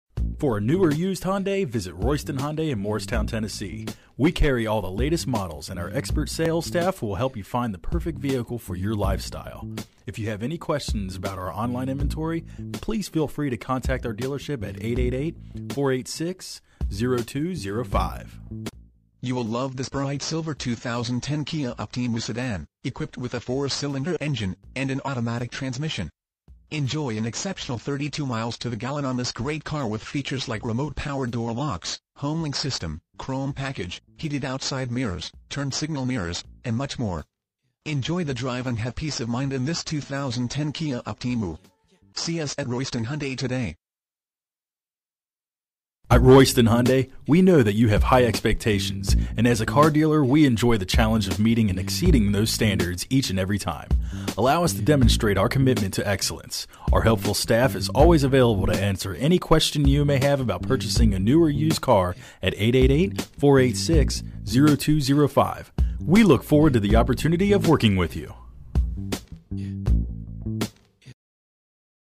2010 Kia Optima Royston Hyundai Morristown, TN 37814
Kia Optima Check out this Bright Silver Kia Optima Sedan, equipped with a 4 Cylinder engine and an automatic transmission. Enjoy an exceptional 32 miles to the gallon on this great car with features like Remote Power Door Locks, Homelink System, Chrome Package, Heated Outside Mirrors, Turn Signal Mirrors, Tire Pressure Monitoring System, on steering wheel audio and cruise controls, and much more. Enjoy the drive and have peace of mind in this Kia Optima. See us at Royston Hyundai today!
MILES: 56,096
VIN:KNAGG4A87A5391123

Royston Hyundai
5524 w. Andrew Johnson Hwy.